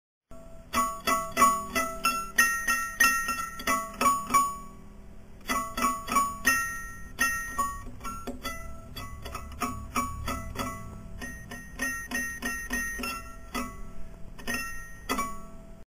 "No" (2009)
I'm not sure why I took this down. The music is extremely high notes on my guitar, Rosalia Lombardo II. This is titled "No", made just last year, 2009.
In it is just a plastic little rat (I love rats!) and myself.
You know, I rarely wear my glasses.